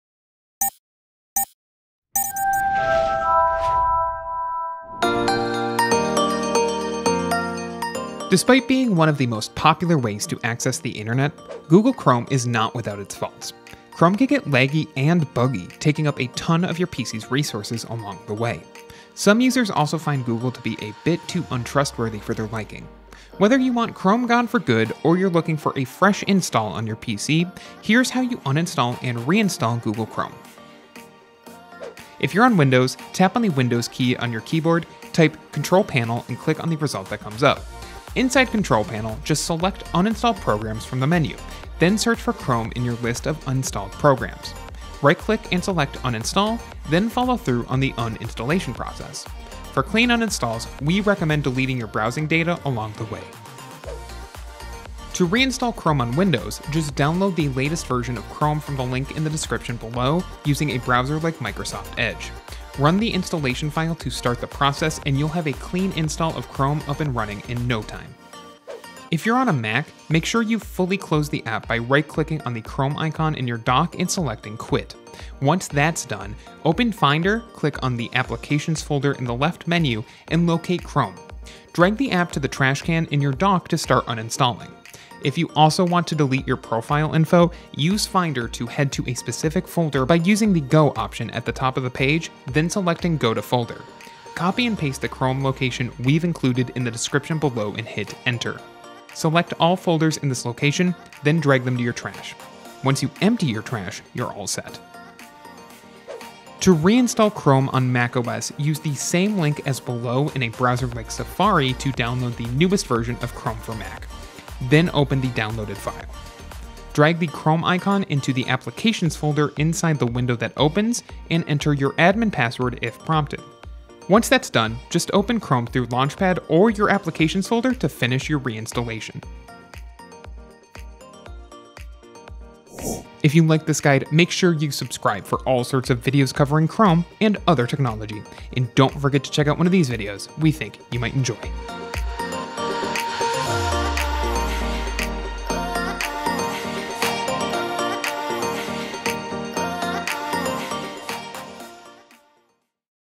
HOW TO Uninstall and Reinstall Google Chrome! (Windows & Mac)
Google Chrome is not without its faults. The world's most popular browser can get laggy and buggy, taking up a ton of your PC’s resources along the way. Some users also find Google to be a bit too untrustworthy for their liking.  Whether you want Chrome gone for good, or you’re looking for a fresh install on your PC, here’s how you uninstall and reinstall Google Chrome.